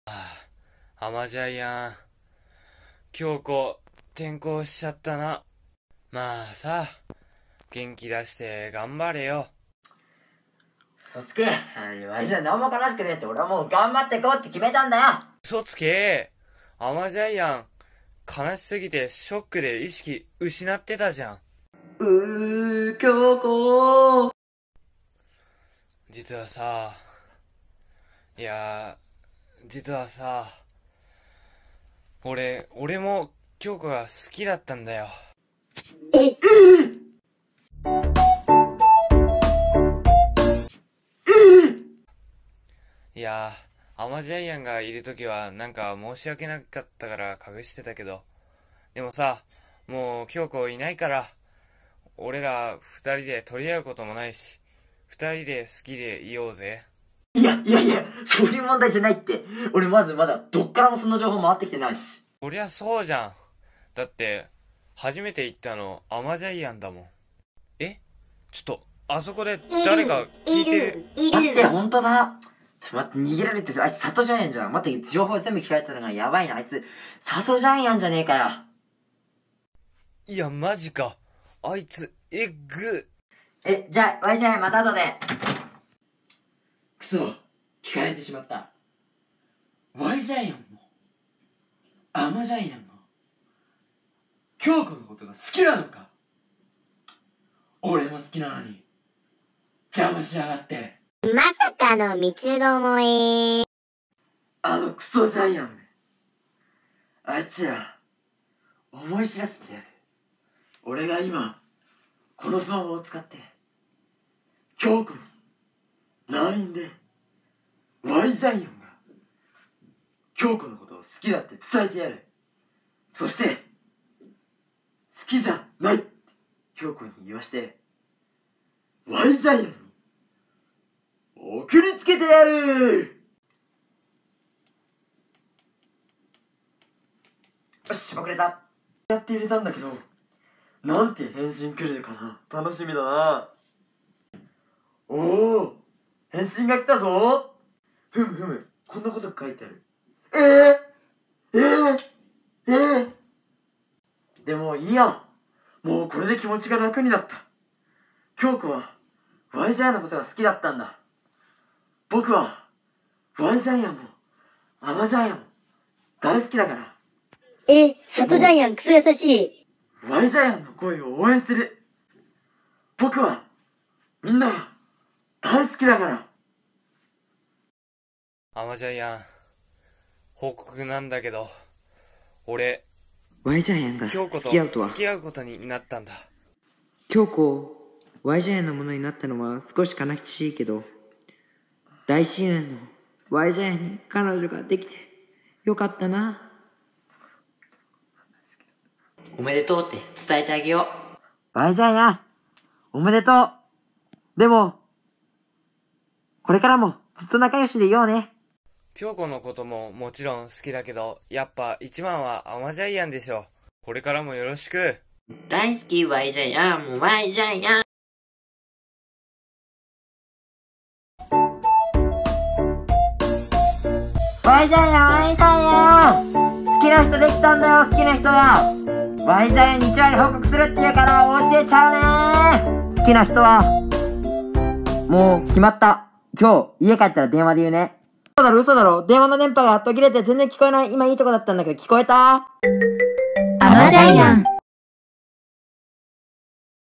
中学生ドラマ 第二話告白編
前回の中学生ドラマ　第一話の続きです

前回告白出来なかったあまジャイアンどうなるのか？